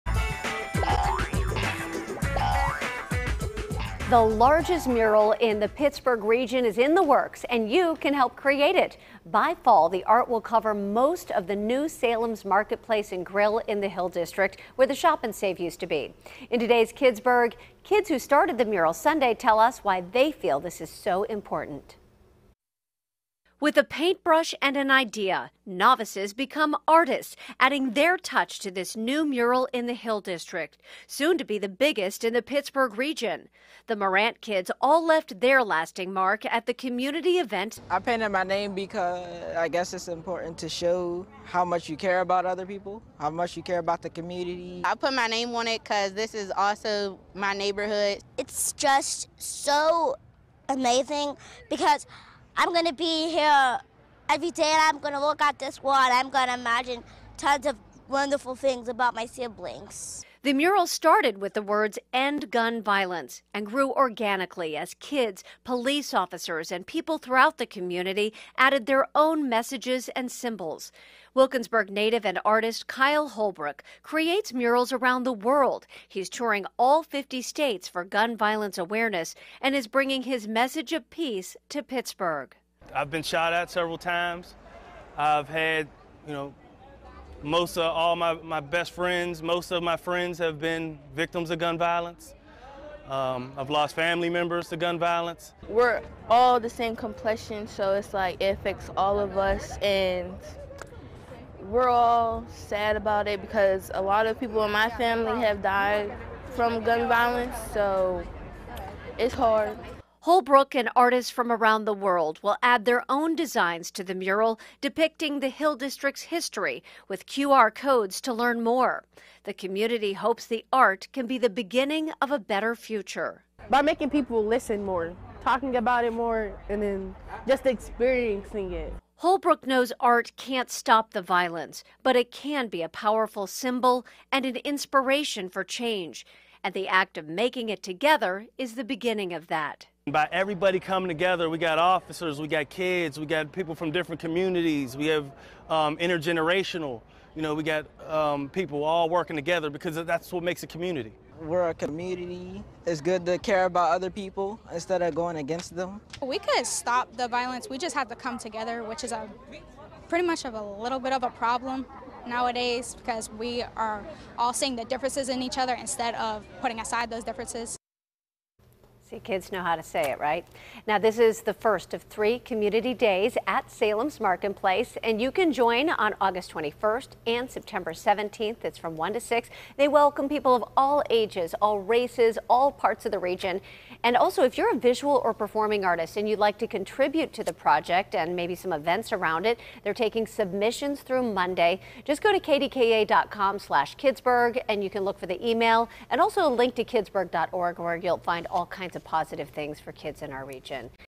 Largest mural in Pittsburgh regions is in the works
The community hopes the art can be the beginning of a better future.